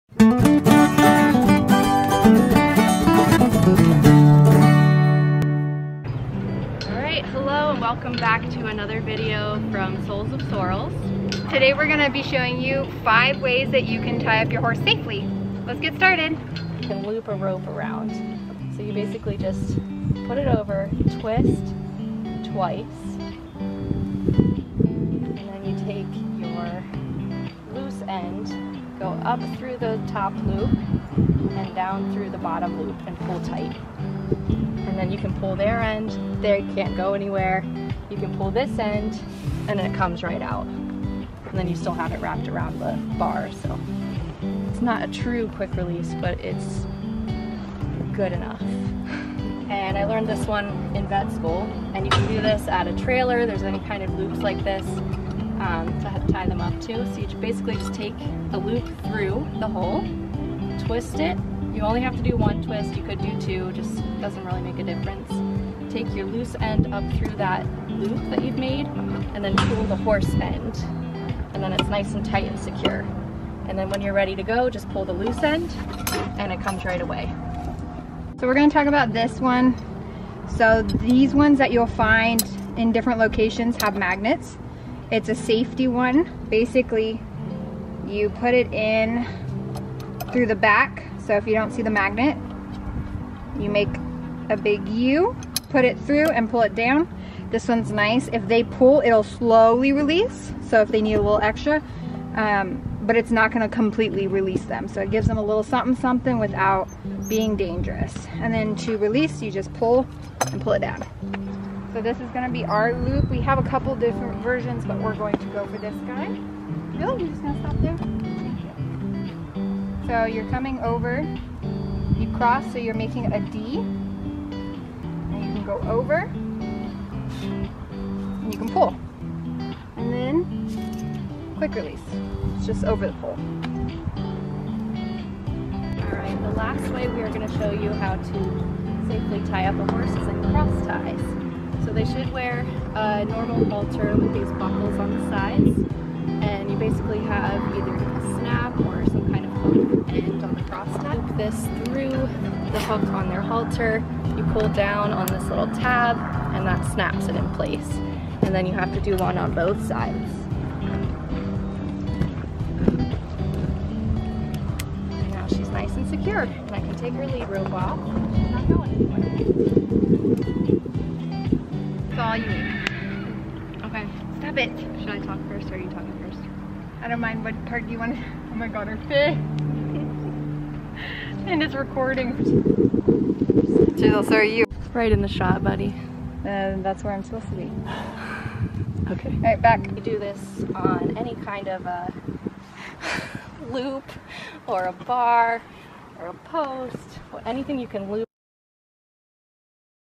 5 Ways to safely tie your horse
In this video we give you 5 ways to safely and securely tie up your horses at different locations! Tell us if use any of the ways!

Check us out on our other socials!